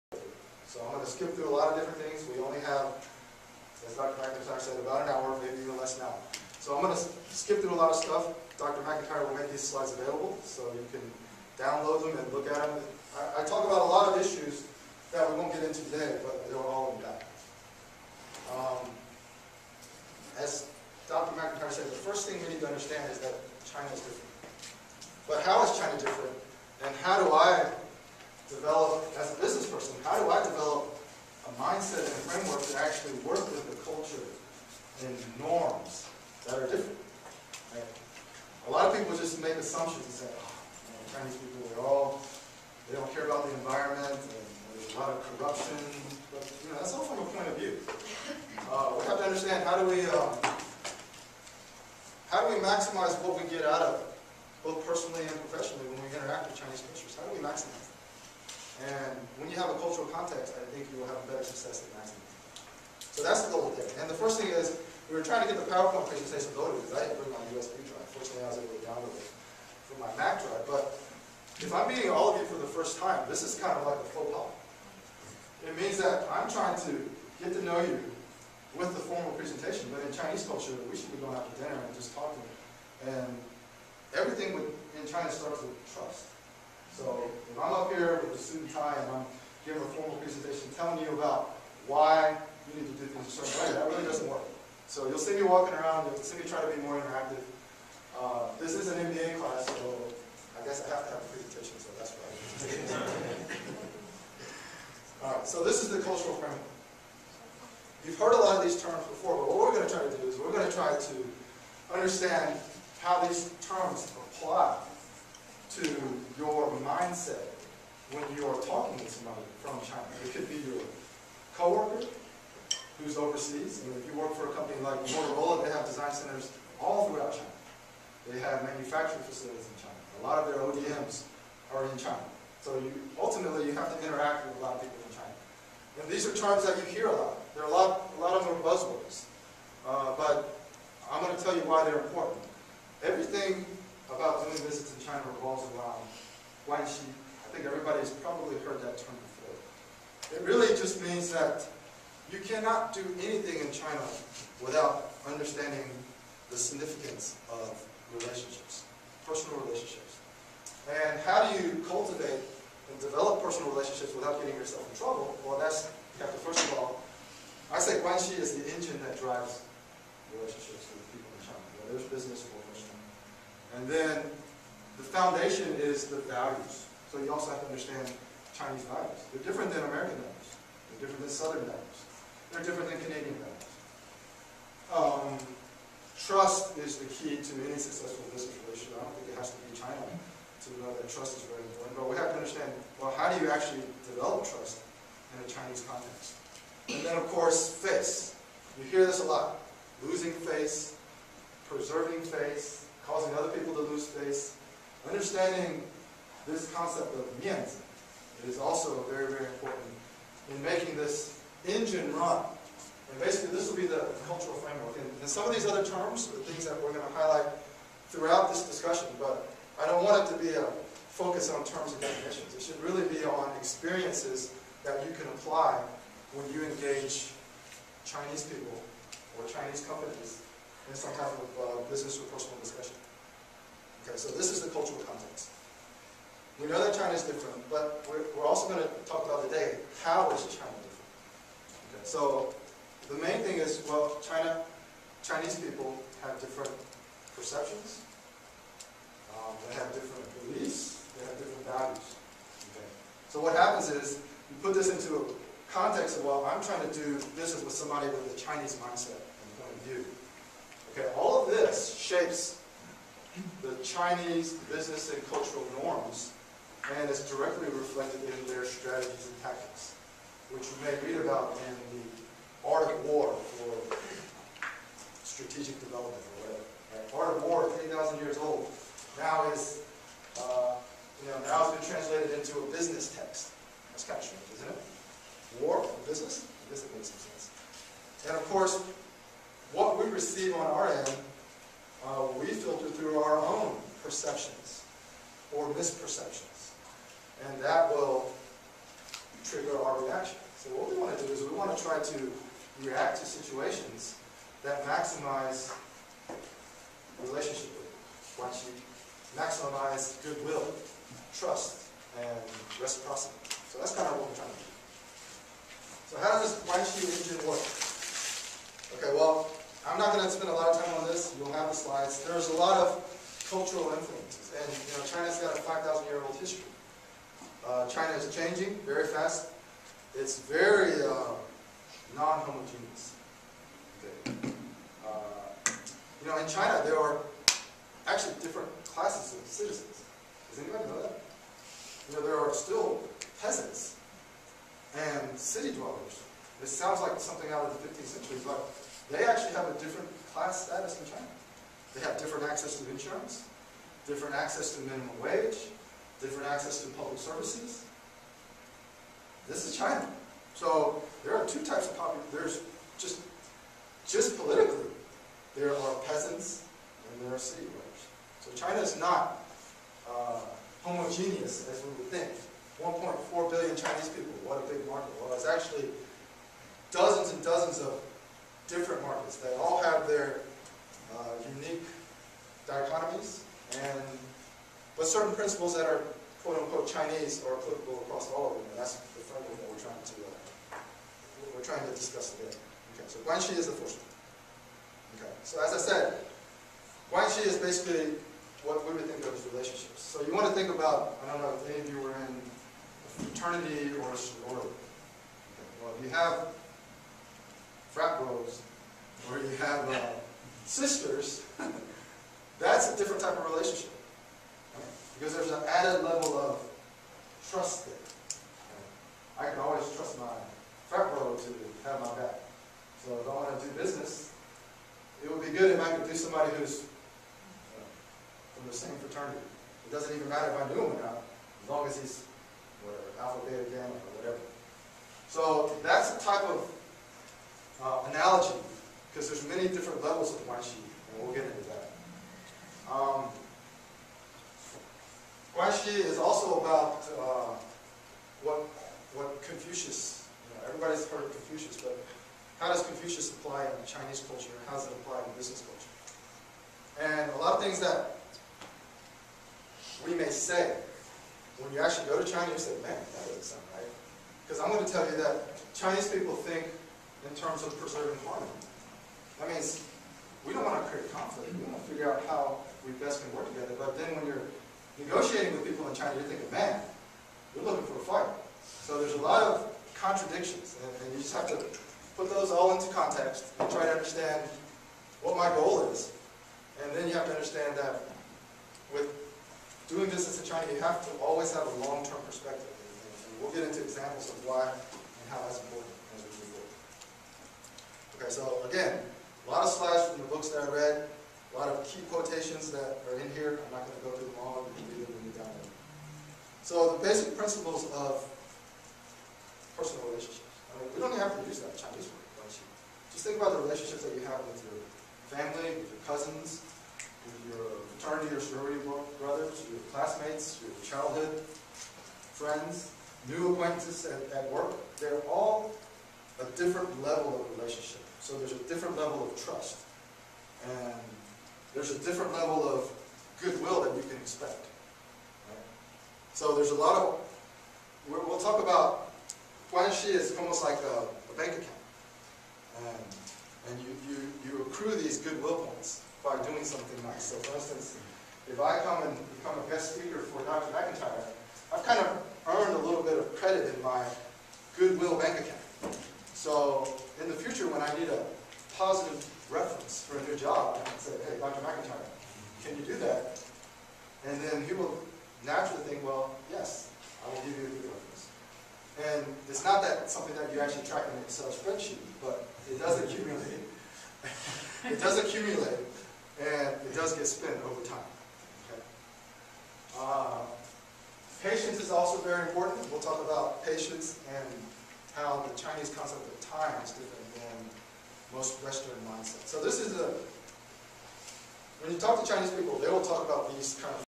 Mindset for China Business: Developing a Cultural Framework (Part 2 of 5)
Part 2 of 5 of a guest speaking appearance on March 31, 2010 at Georgia Tech College of Management for MBA students on developing a cultural framework and mindset for doing business in China.  The class is MGT6185 Global Business Environments.  The professor is Dr. John McIntyre, Director, CIBER GT.